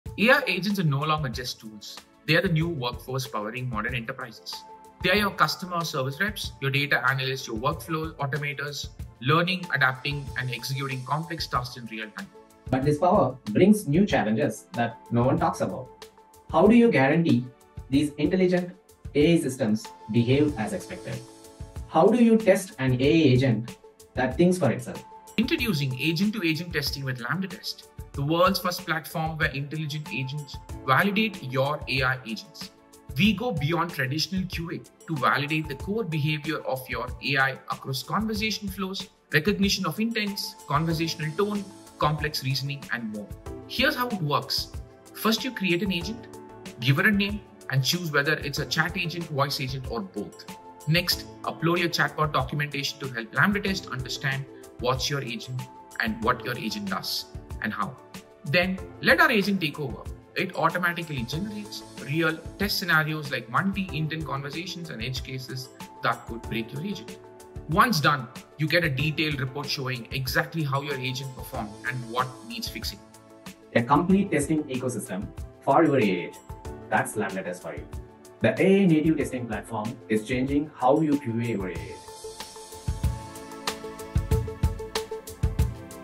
Launching the World’s First Agent to Agent Testing Platform
We’re excited to announce the launch of LambdaTest’s Agent to Agent Testing Platform, the first of its kind in the industry! 🎉

The platform enables comprehensive testing for chatbots, voice assistants, and autonomous systems, ensuring accuracy, reliability, and faster releases. By using AI agents to test other AI agents, you can validate complex real-world scenarios that traditional methods simply cannot handle.

𝐖𝐢𝐭𝐡 𝐋𝐚𝐦𝐛𝐝𝐚𝐓𝐞𝐬𝐭’𝐬 𝐀𝐠𝐞𝐧𝐭-𝐭𝐨-𝐀𝐠𝐞𝐧𝐭 𝐓𝐞𝐬𝐭𝐢𝐧𝐠 𝐩𝐥𝐚𝐭𝐟𝐨𝐫𝐦, 𝐲𝐨𝐮 𝐜𝐚𝐧:
✅ Automate complex AI system validation
✅ Achieve faster feedback loops for rapid iteration
✅ Ensure greater test coverage and reliability
✅ Reduce costs while maintaining quality

Start testing smarter with LambdaTest today.